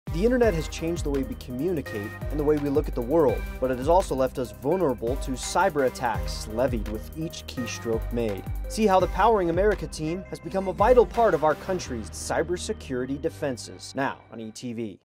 The Powering America Team Takes on Cyber Security Safety and Training at New Facility
Cyber criminals and the damage they inflict can cripple businesses, commerce, and finance. The Powering America Team of NECA/IBEW is helping to build a new facility to help train security teams, develop infrastructure, and deploy defenses in this regard. The Georgia Cyber Innovation and Training Center will play an instrumental part in our country’s safety moving forward.